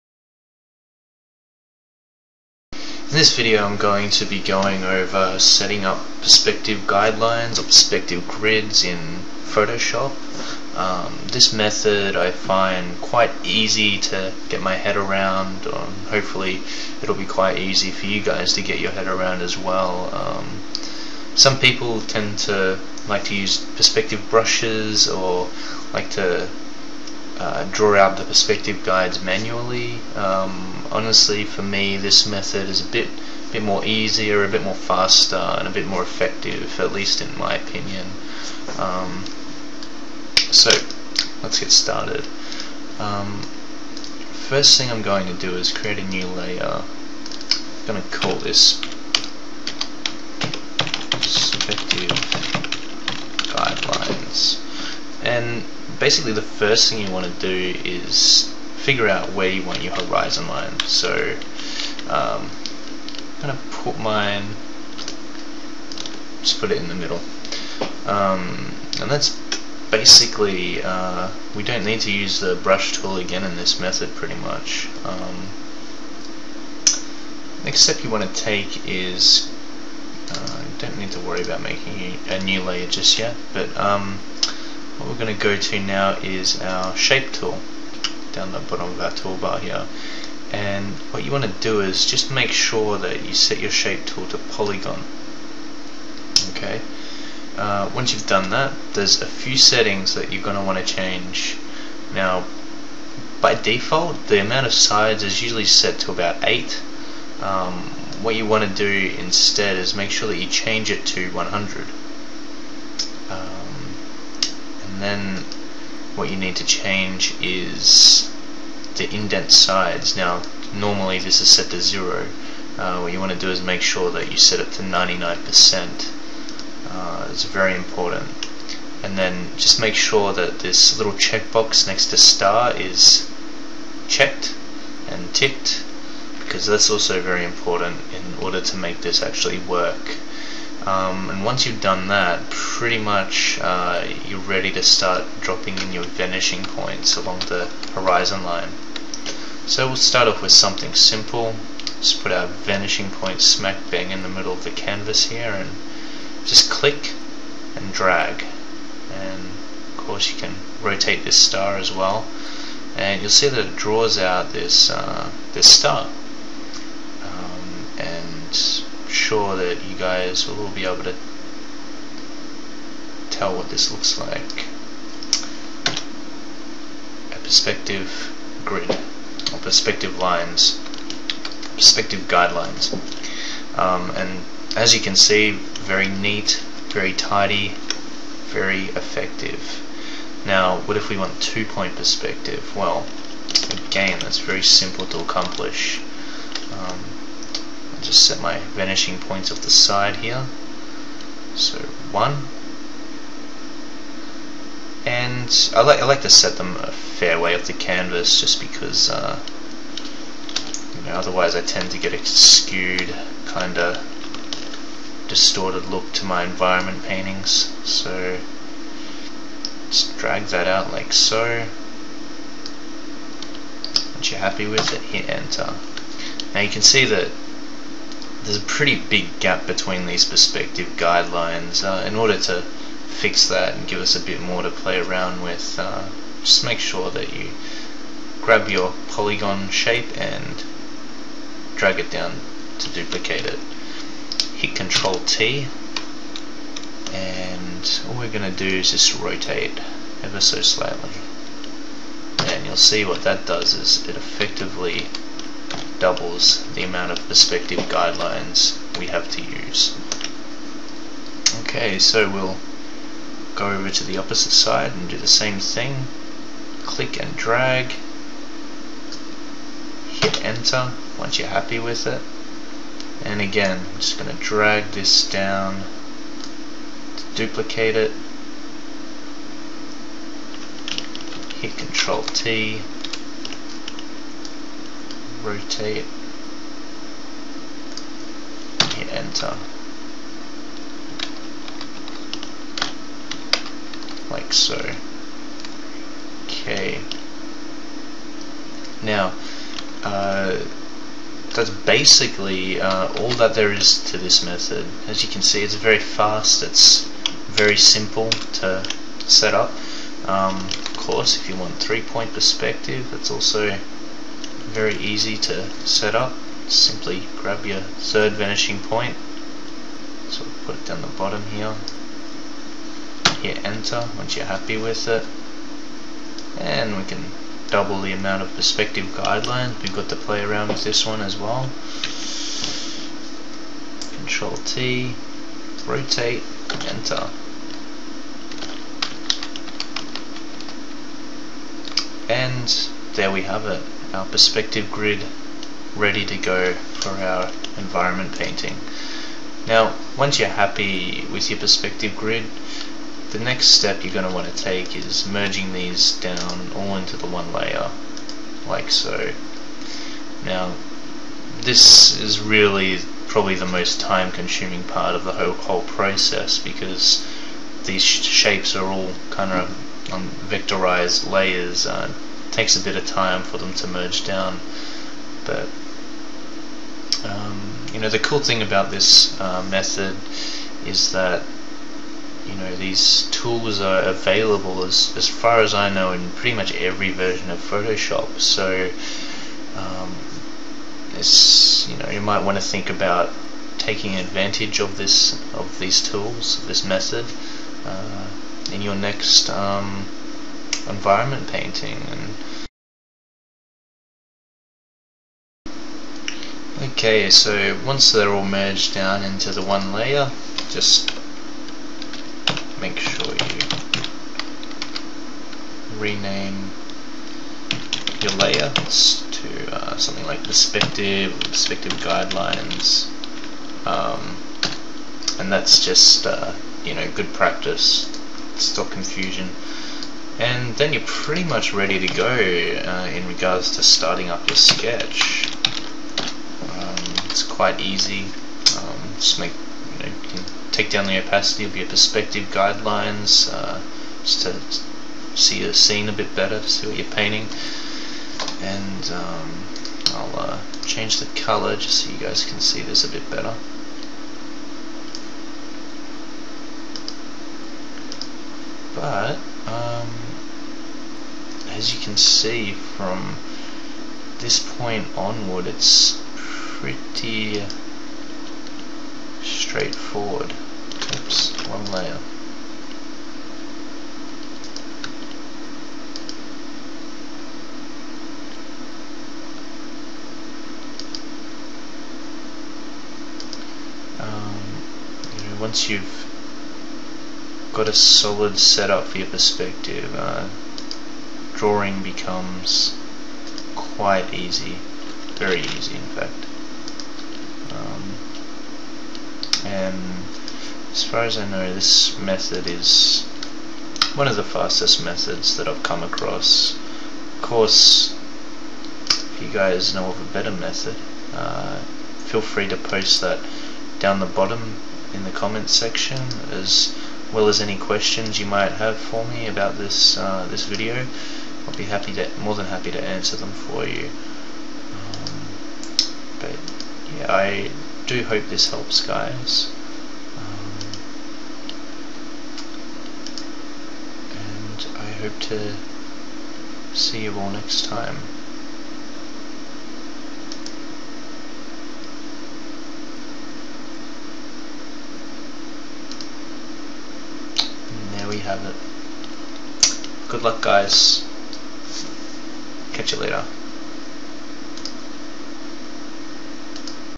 Perspective Setting up Perspective Grids In Photoshop
In this tutorial I demonstrate how to easily and effectively set up a perspective grid inside Photoshop.